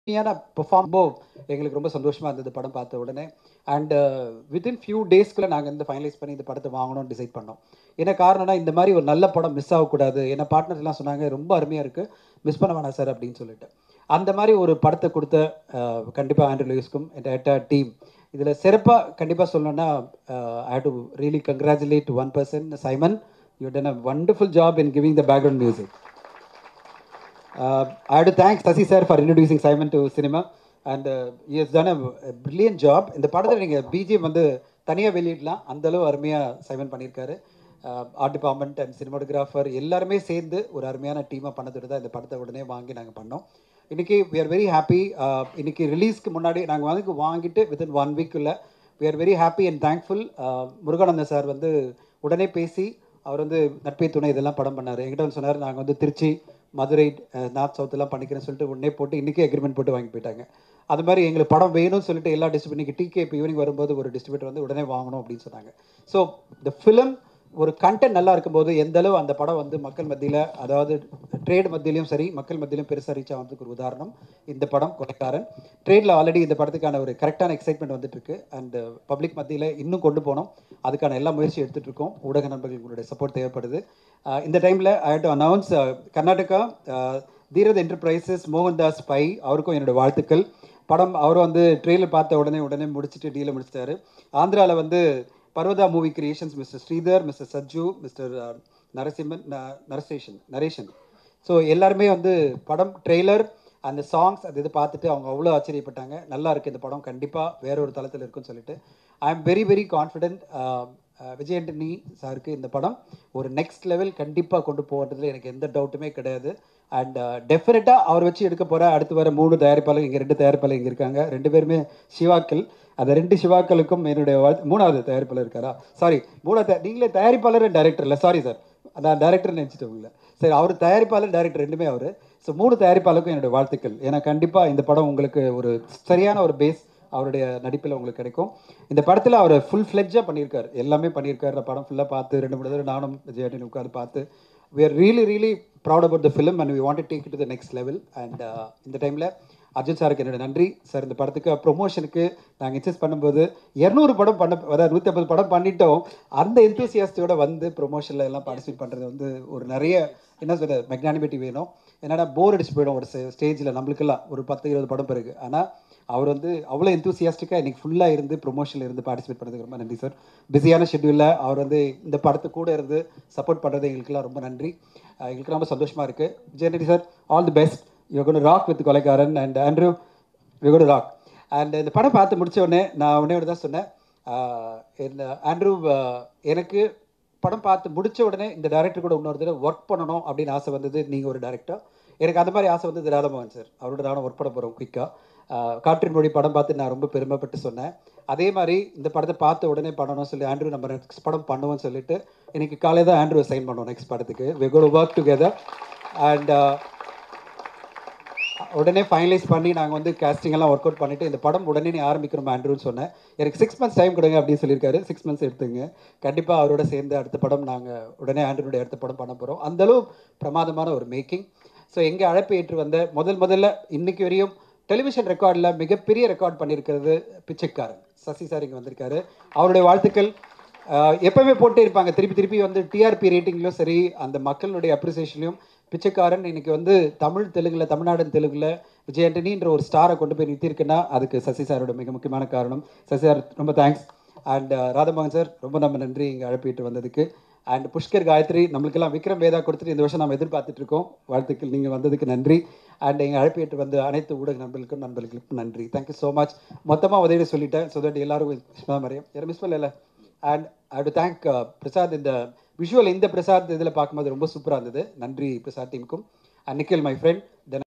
Dhananjayan speech at Kolaigaran movie audio launch | Arjun, Vijay Antony, Ashima Narwal| STV
LIVE | Lok Sabha Elections 2019 Announcement |EC to announce polling dates| BJP|CONGRESS|AIDMK |DMK